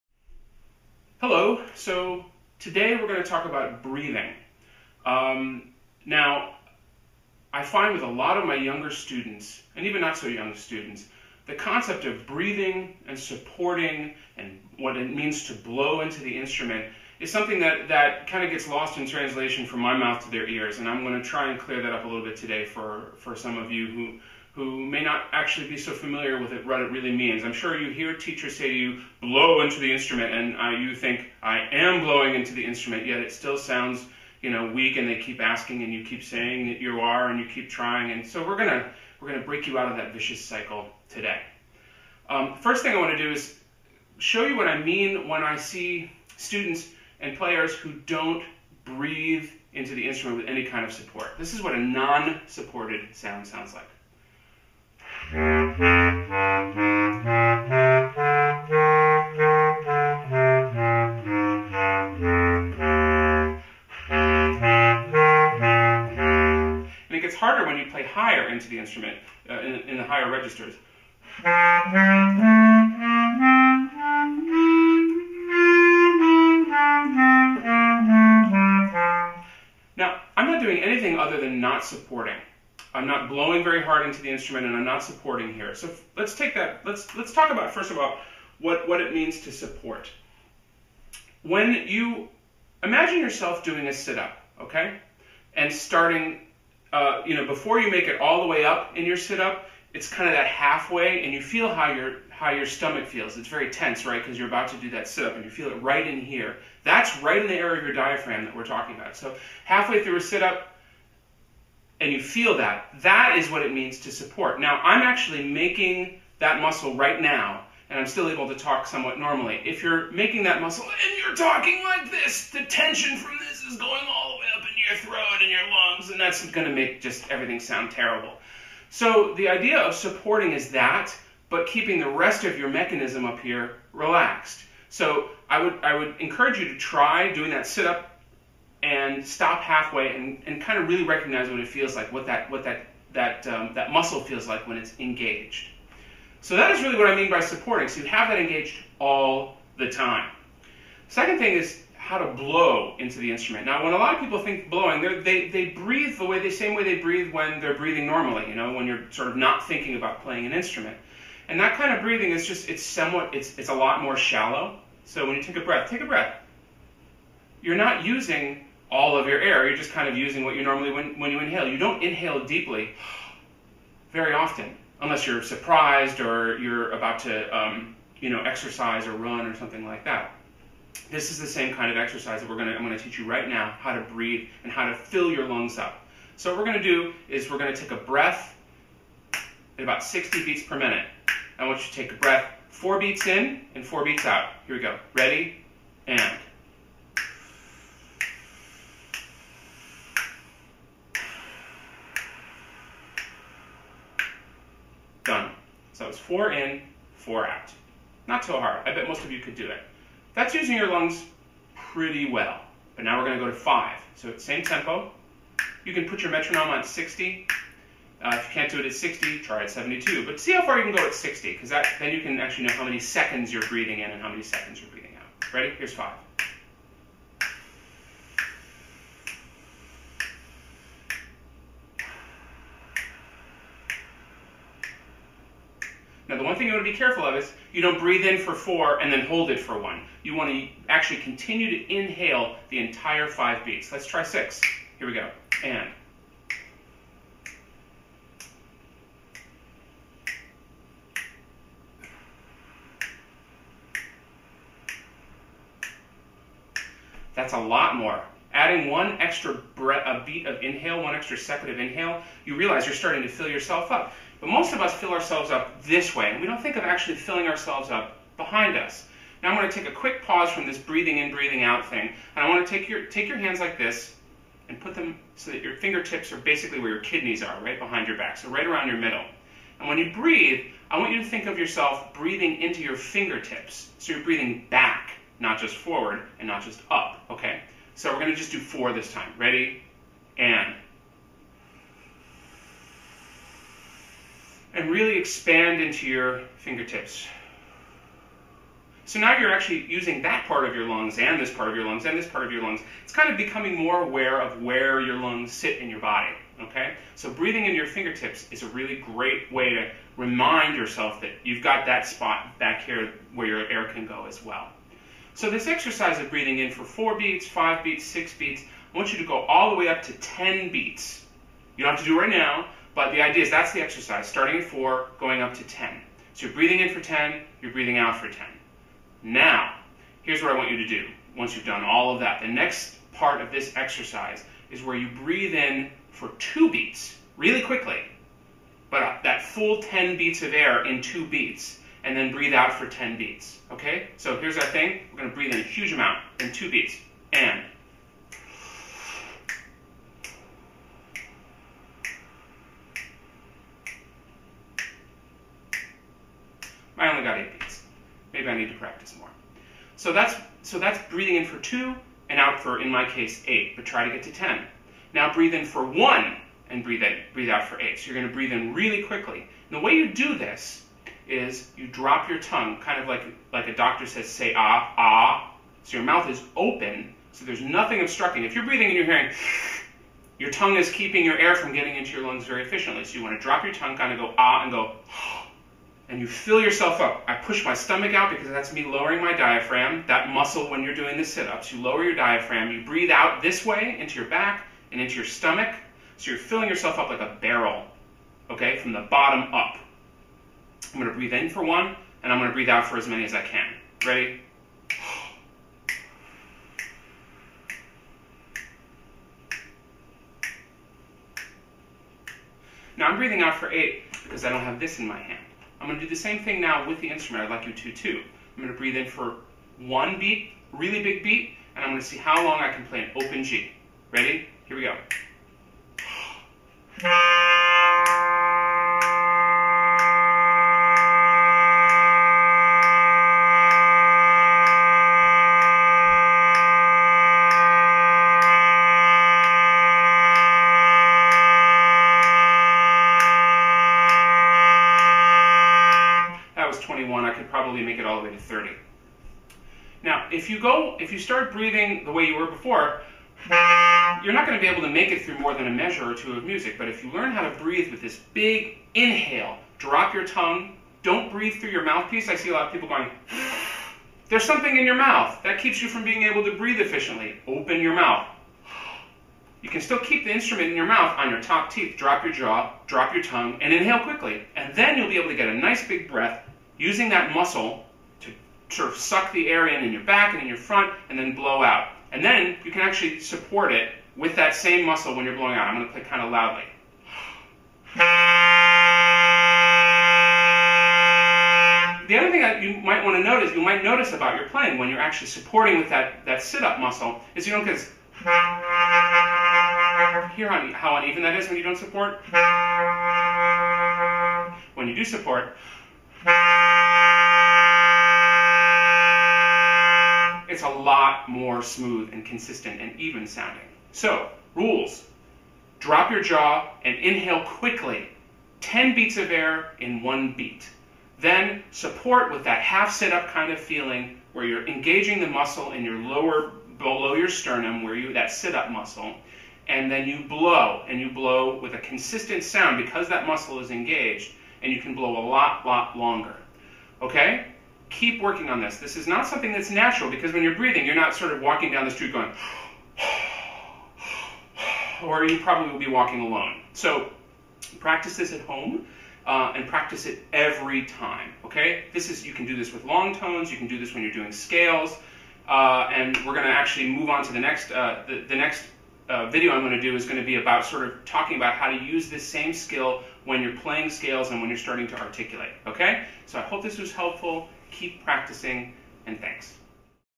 How to get a good sound a Bass Clarinet (or clarinet, or sax, or...)
Want a good sound? Nice consistent tone? Do you want to be able to play more than 2 measures without having to take a breath? Do you sound like a 94-year-old grandmother playing the clarinet?

It's all about how you BREATHE, and SUPPORT your air. This video will show you a few tricks to get there.